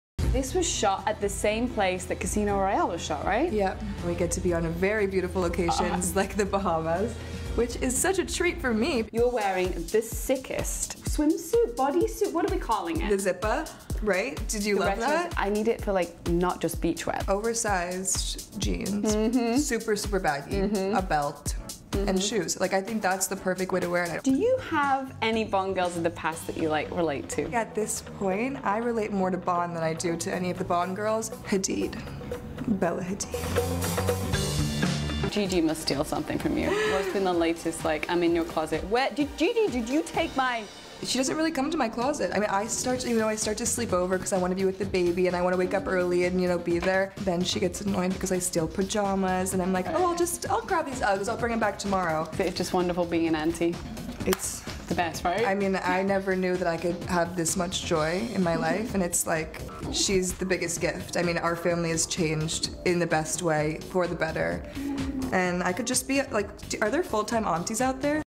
Bella on Bond, bags and babies. Watch her E! Interview with Zanna Roberts Rassi.
Bella on Bond, bags and babies. Watch her E! Interview with Zanna Roberts Rassi. MMKx007 @edailypop @enews